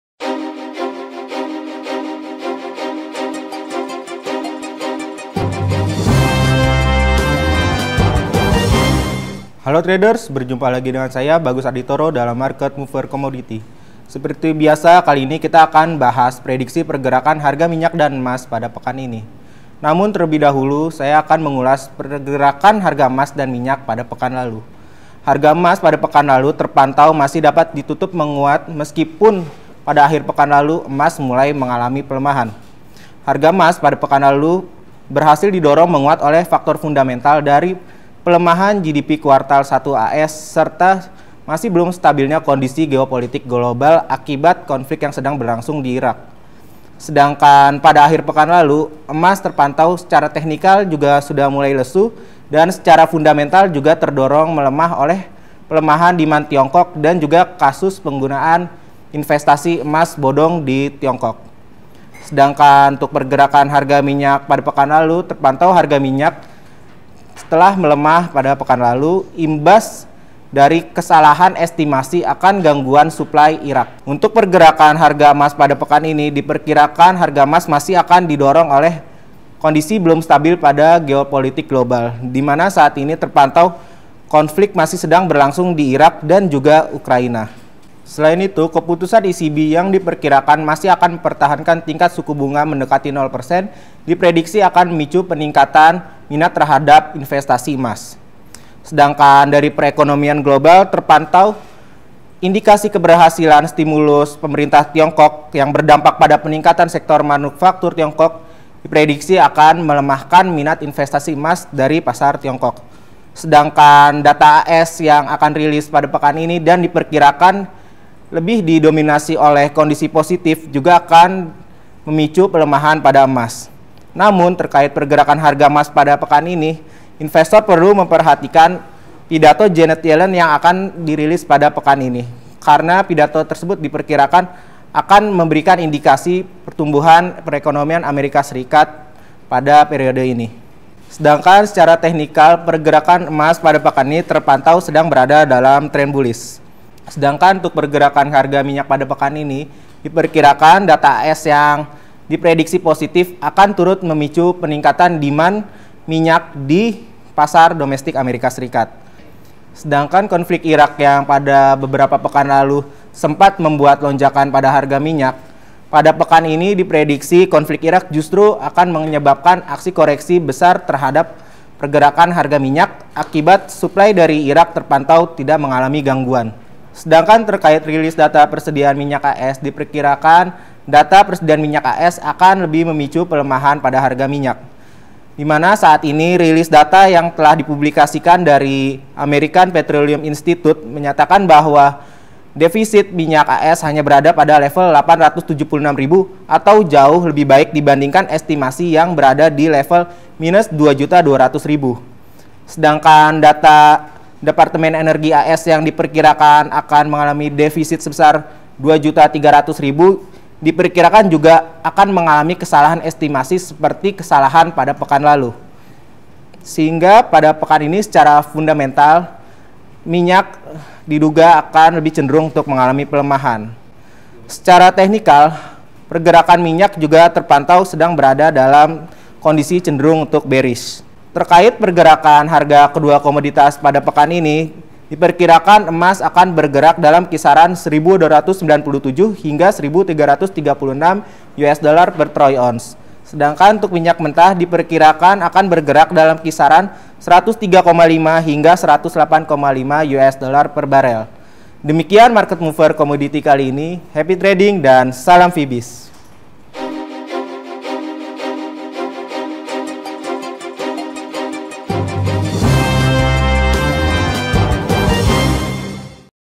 Bursa Komoditas Tunggu Rentetan Data AS, Vibiznews 2 Juli 2014
Program : Market Mover Commodity by Vibiznews

Program Market Mover Commodity edisi pekan ini, analis Bagus Aditoro mengulas pergerakan harga emas dan minyak mentah pekan lalu serta prediksi untuk pekan ini. Harga minyak diprediksi akan melemah akibat dampak tidak terganggunya supply minyak mentah Irak serta potensi over estimasi data persediaan minyak AS. sedangkan harga emas berpotensi untuk menguat akibat belum stabilnya kondisi geopolitik global. namun, secara keseluruhan, pergerakan harga emas dan minyak akan sangat dipengaruhi oleh rentetan rilis data perekonomian AS pada tengah pekan ini.